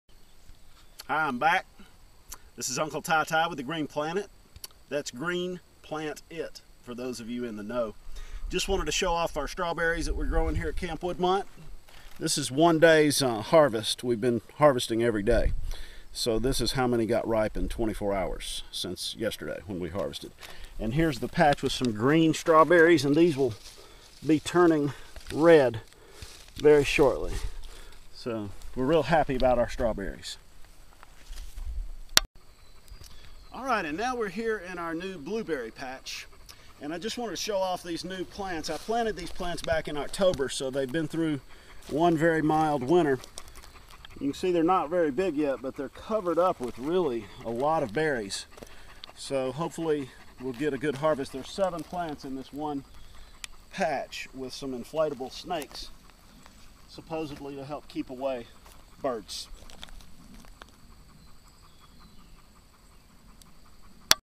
berries berries berries !!
Strawberries and blueberries at Camp Woodmont 5/1/2012